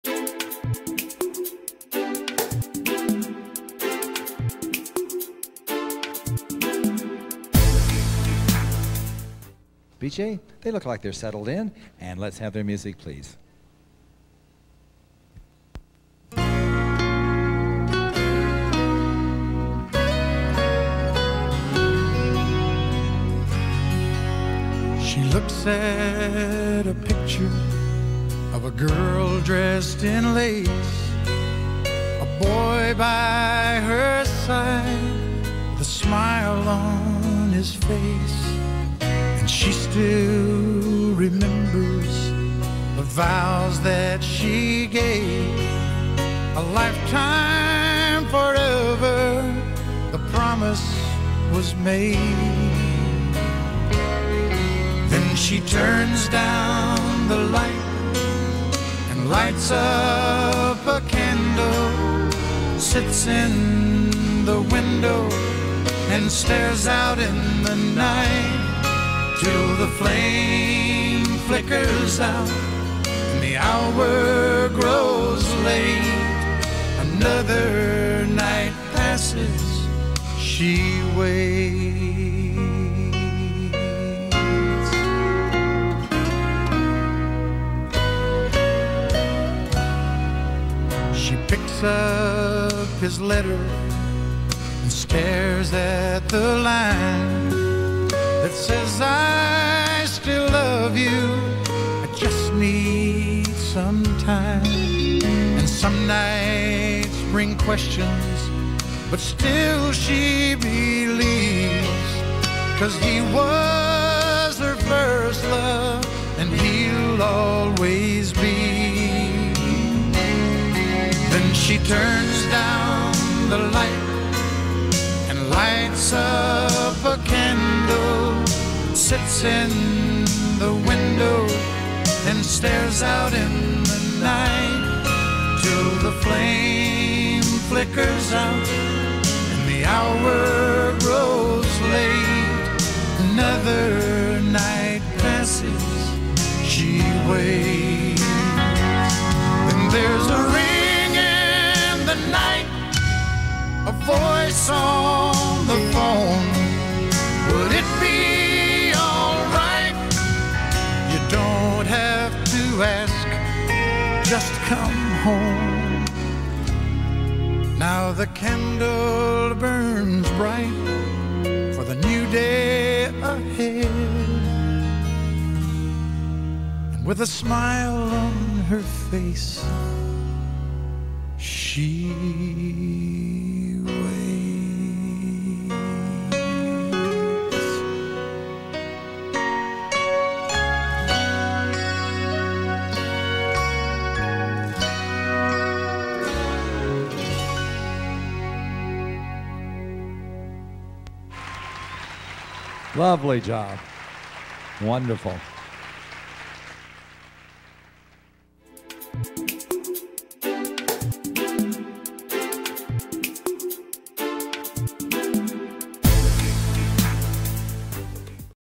Wayne Chapman | Annette Chapman | Waltz | Showcase Division Two | 1996 Colorado Country Classic
Wayne Chapman | Annette Chapman | Waltz | Showcase Division Two | 1996 Colorado Country Classic | Denver, Colorado